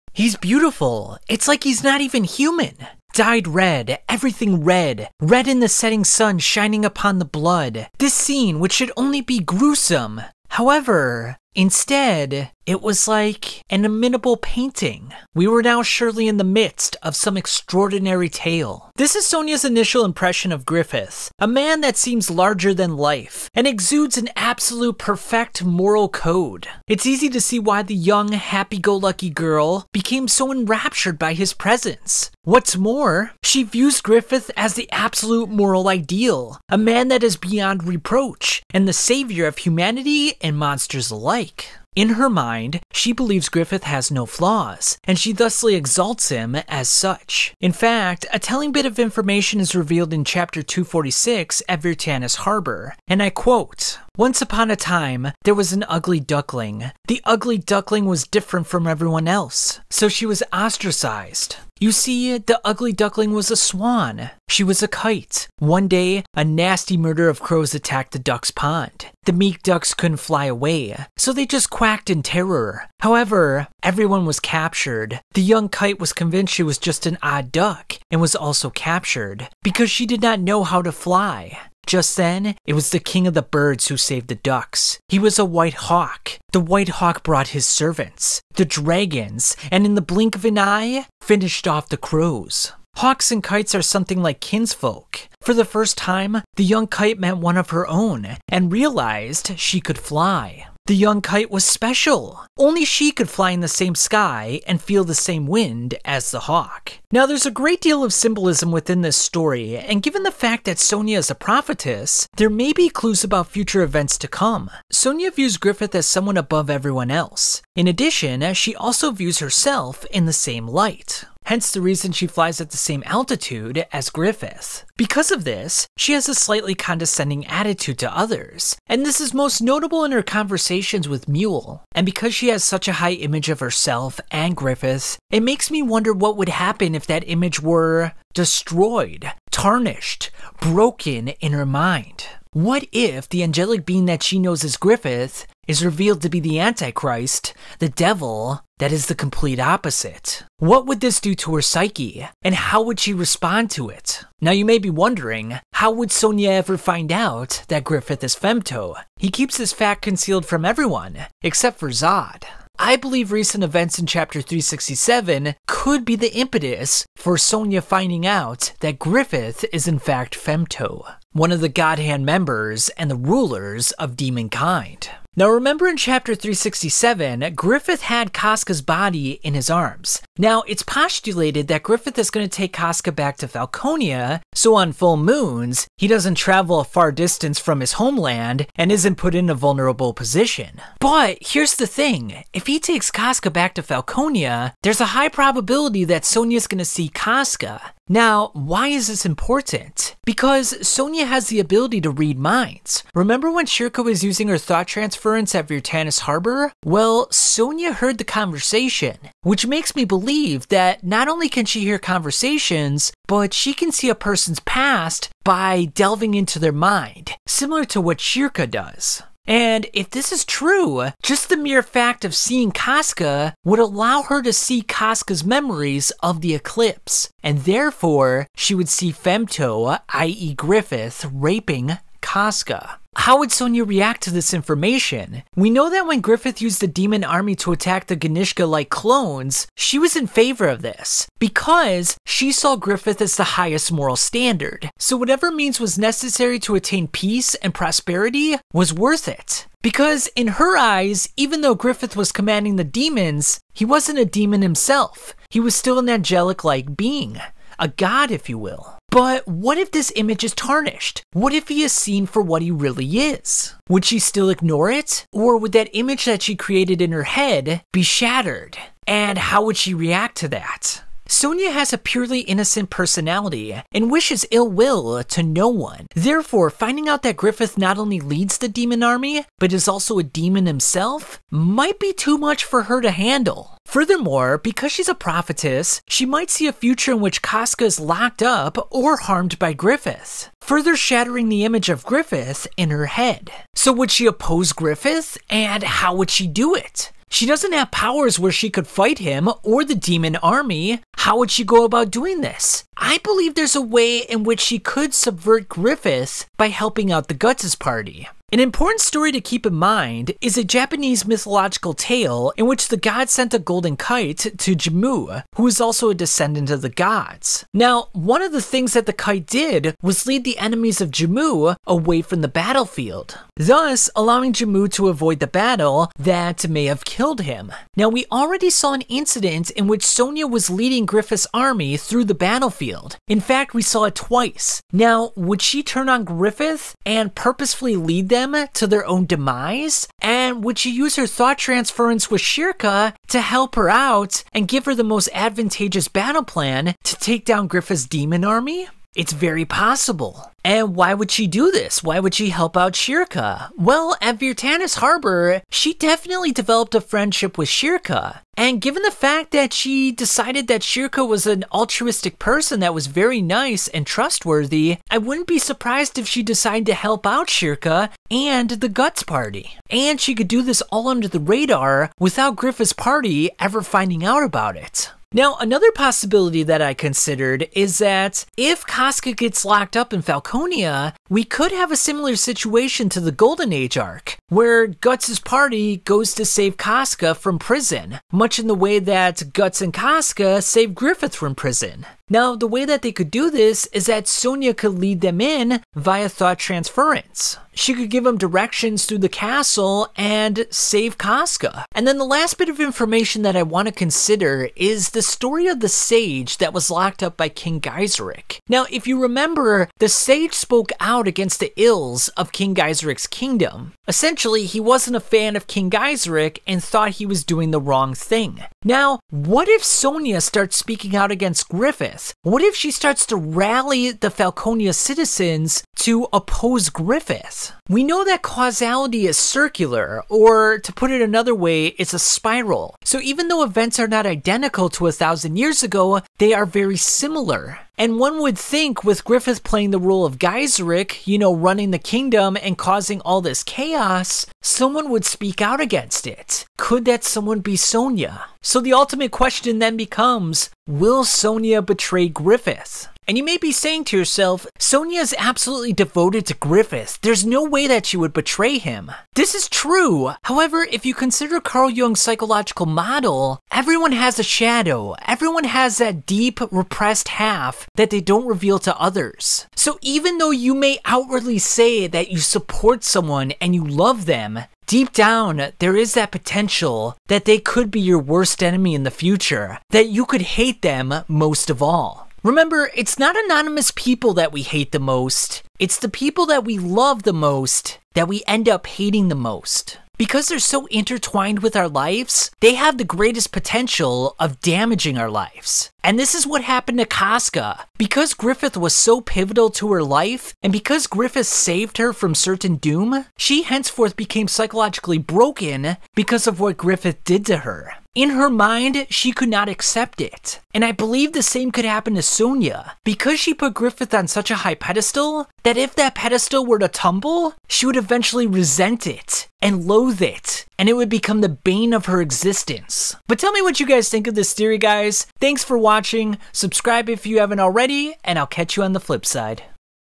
Will Sonia Betray Griffith [Berserk 368 speculation]
Berserk 368 is coming soon, and this shake the very core of Griffith's army?